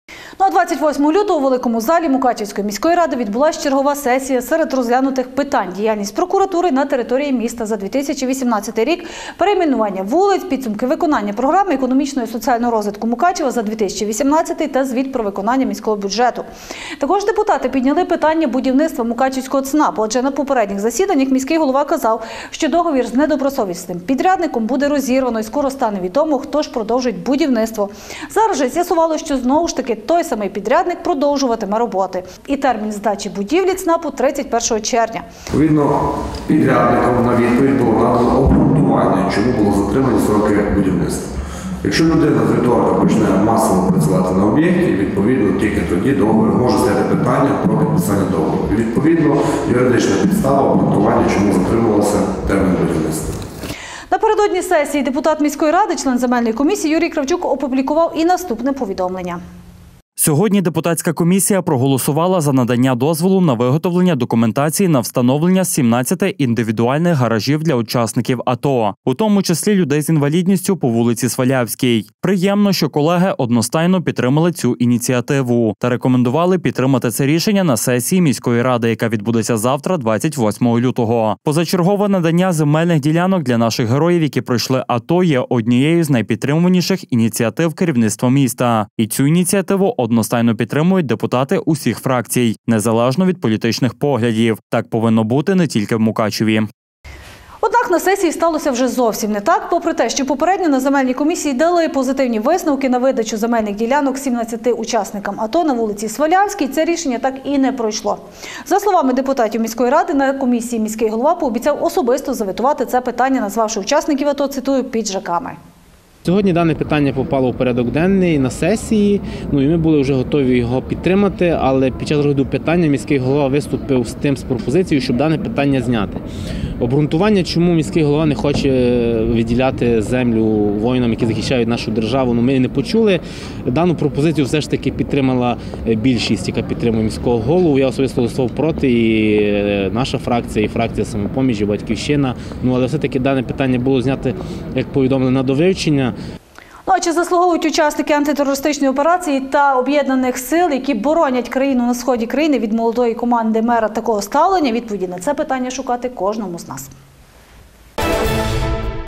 Міська сесія
Виготовити документи на встановлення 17 гаражів для учасників АТО, у тому числі інвалідів, на вулиці Свалявській в Мукачеві вирішила дозволити депутатська комісія міськради. Однак на сесії це рішення так і не пройшло. За словами депутатів міської ради, на комісії мер пообіцяв особисто заветувати це питання, назвавши учасників АТО «піджаками».